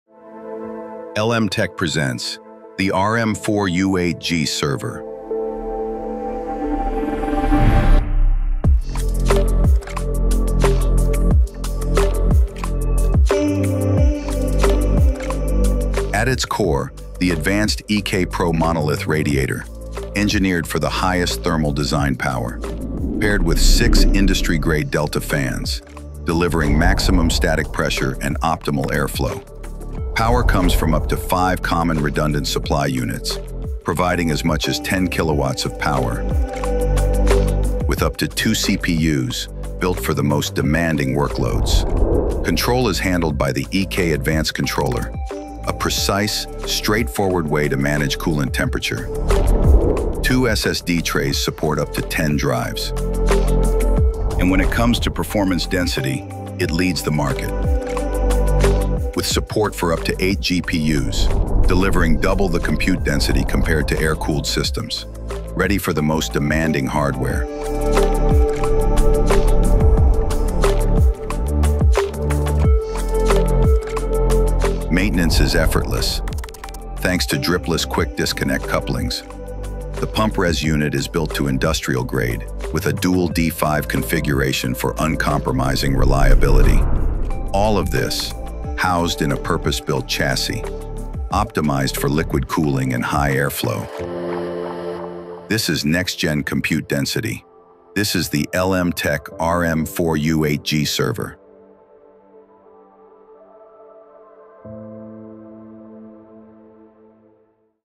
LM TEK RM-4U8G Server | Next-Gen Liquid-Cooled Compute Density
Introducing the LM TEK RM-4U8G Server — engineered for maximum density, maximum performance, and uncompromising reliability.

Featuring:
✅ Advanced EK-Pro radiator for highest Thermal Design Power
✅ Six industry-grade Delta fans for optimal flow and pressure
✅ Up to 5 redundant power supplies delivering 10kW with N+1 redundancy
✅ Optimized cooling pathways for maximum CPU performance
✅ EK Advanced Controller with hardware-level safeguards
✅ Dual D5 industrial-grade pump-res unit
✅ Support for up to 8 GPUs – delivering double the compute density of air-cooled systems
✅ Easy maintenance with dripless quick-disconnect couplings
✅ Purpose-built chassis for liquid cooling, airflow, and seamless upgrades

Built for AI, HPC, and next-generation workloads, the RM-4U8G is sustainable, upgradable, and future-proof.